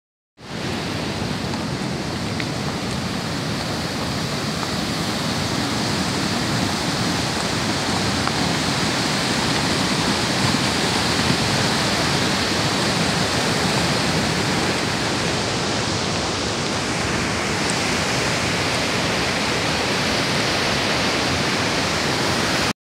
Horseback riding in Kyrgyzstan
Riding horses up to the glaciers